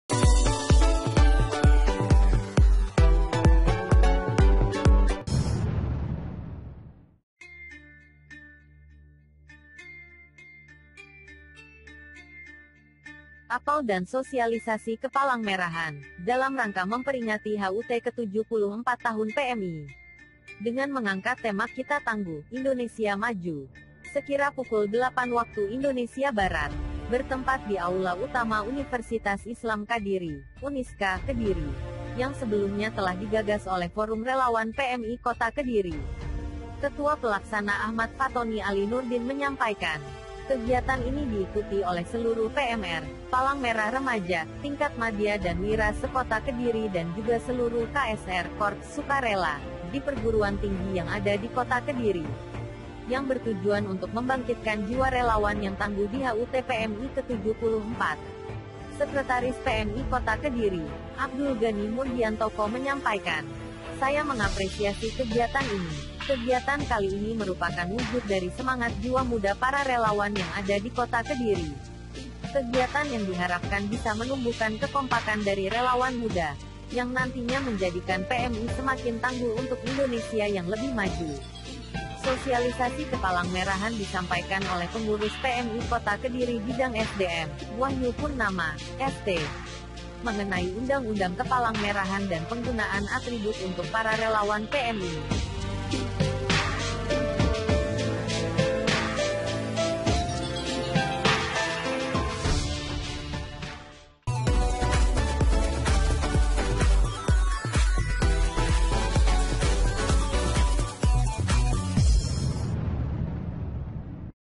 Palang Merah Indonesia (PMI) HUT ke-74 Kota Kediri Gelar Sosialisasi ...
Palang Merah Indonesia (PMI) Kota Kediri Gelar Sosialisasi Kepalangmerahan Peringati HUT ke-74

Palang Merah Indonesia (PMI) Kota Kediri menggelar apel dan sosialisasi kepalangmerahan, dalam rangka memperingati HUT ke-74 tahun PMI dengan mengangkat tema "Kita Tangguh, Indonesia Maju"...